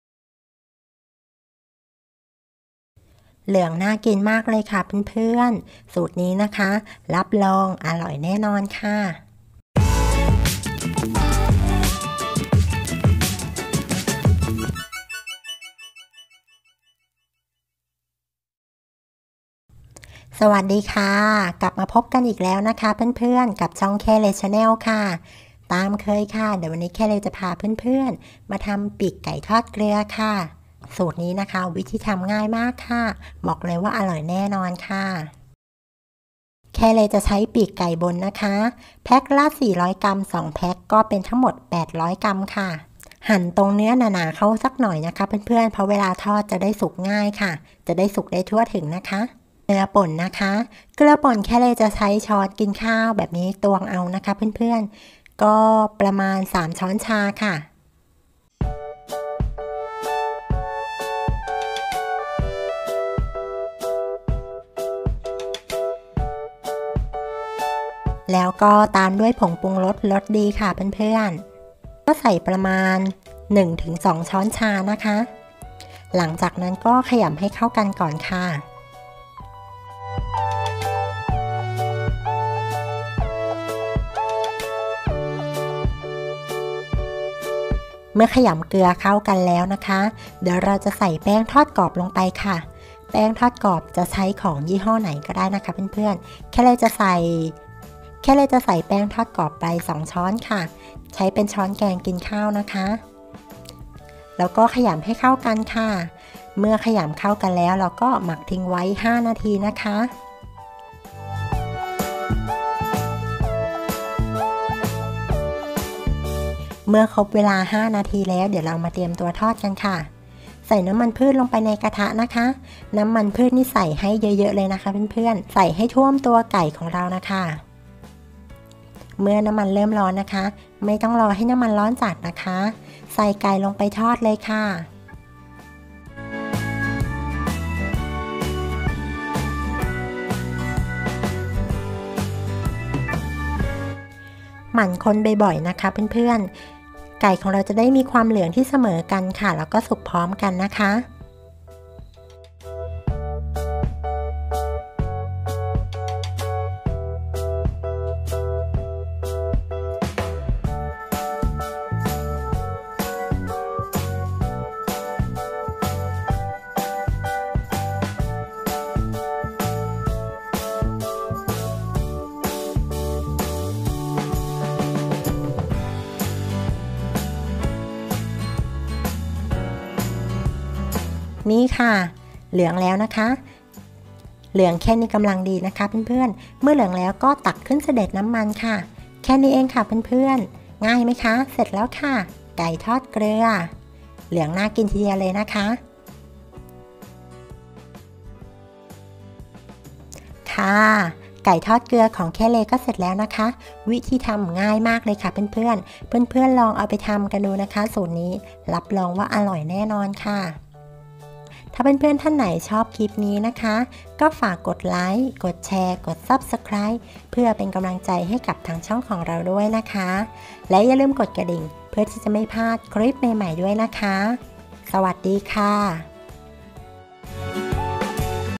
ปีกไก่ทอดเกลือ​ หมักให้อร่อยแค่​ 5​นาที​ กรอบนอกนุ่มใน​ สูตรนี้ลงตัวมาก​ ทำขายได้​ ep91|แค่เลchannel
ปีกไก่ทอดเกลือ​ สูตรนี้เป็นสูตรที่ลงตัวมาก​ วิธีหมักไก่ให้กรอบอร่อยแค่​ 5​ นาที แค่เลจะใช้​ ปีกบนไก่​ นำมาทอด​ สูตรนี้ใช้ได้ทั้งกับเนื้อไก่​ เนื้อหมู​ เนื้อ​ ปีกไก่ทอดเกลือ​ สูตรที่แค่เลทำสามามารถนำไปขายสร้างอาชีพได้ด้วย​ สูตรนี้อร่อยมากค่ะ

วัตถุดิบ/ส่วนผสม
1.ปีกบนไก่​ 800  กรัม
2.เกลือป่น​ 3​ ช้อนชา
3.รสดี​ 2​ ช้อนชา
4.น้ำมันพืชสำหรับทอด